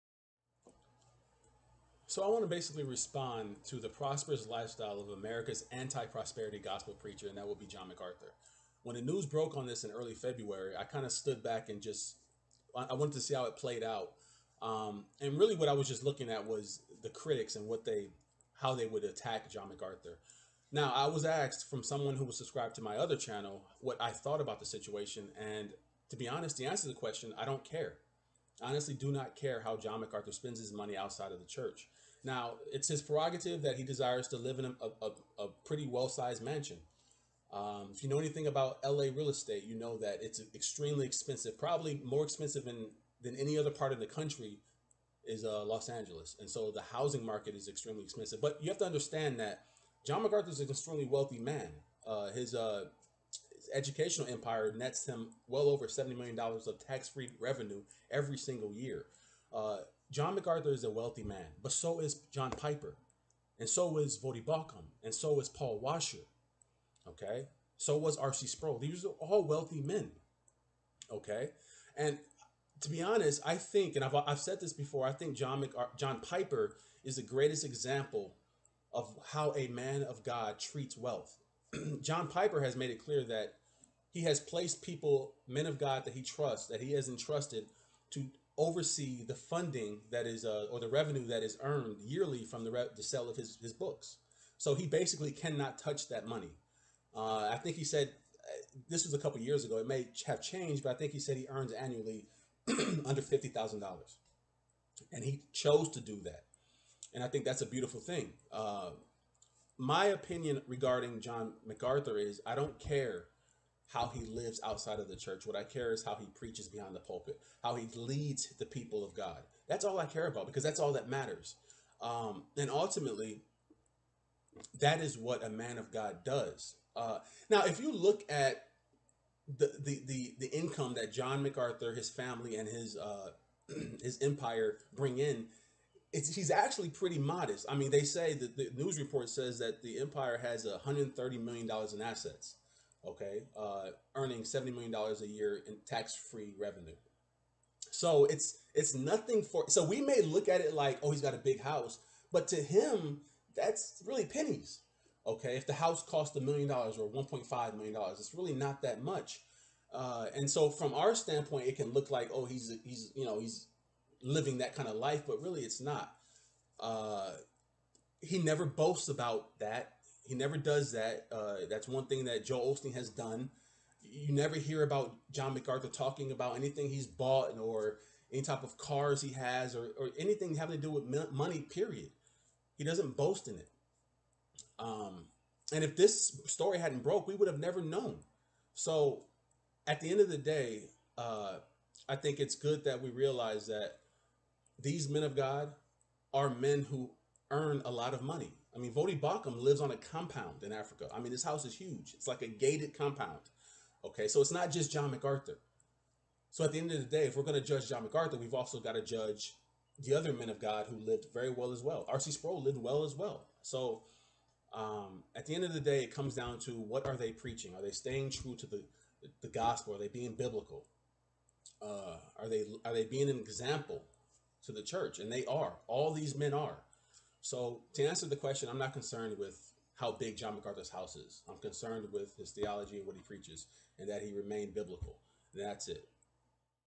My Personal Thoughts On John Macarthur's "Lavish" Lifestyle...
John Fullerton MacArthur Jr. (born June 19, 1939) is an American pastor and author known for his internationally syndicated Christian teaching radio and television program Grace to You.[1] He has been the pastor-teacher of Grace Community Church in Sun Valley, California, since February 9, 1969.[2] He is also the chancellor emeritus of The Master's University in Santa Clarita, California, and The Master's Seminary in Los Angeles, California. MacArthur is a Reformed Protestant and a strong proponent of expository preaching.[3] He has been acknowledged by Christianity Today as one of the most influential preachers of his time[4] and was a frequent guest on Larry King Live as a representative of an evangelical Christian perspective.[5] MacArthur has written or edited more than 150 books, most notably the MacArthur Study Bible, which has sold more than 1 million copies and received a Gold Medallion Book Award.[6]